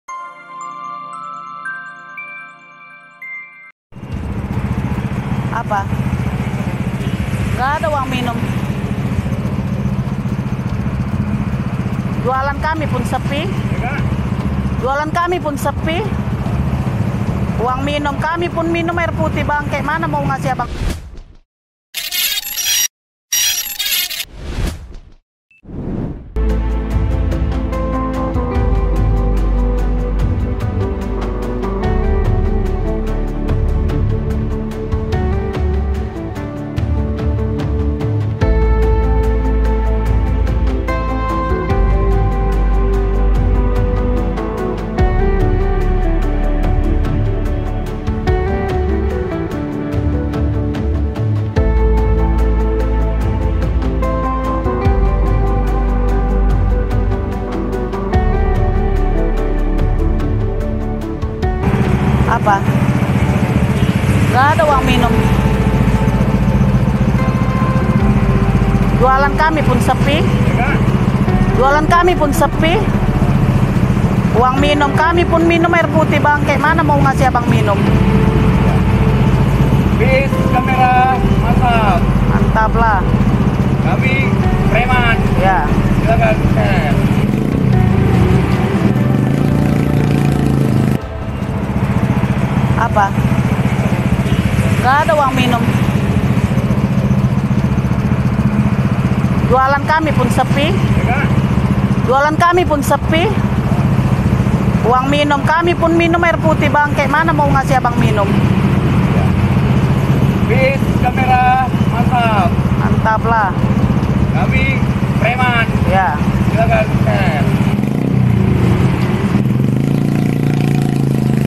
Preman Tengil Minta Jatah Uang ke Pedagang
Beredar sebuah rekaman video dari pedagang buah pinggir jalan yang resah dipalak,

Dua orang yang diduga preman ini berboncengan dan mendatangi pedagang buah.

Bukan sekali dua kali para pengganggu ini datang, menurut informasi pedagang, hari sebelumnya mereka memaki-maki.

Kali ini mereka meminta uang untuk minuman kepada pedagang buah.

Pedagang yang sudah sangat resah meminta kepada pihak berwajib untuk bertindak.

Dengan sok jago mereka mengatakan "poss kamera, kami preman silahkan share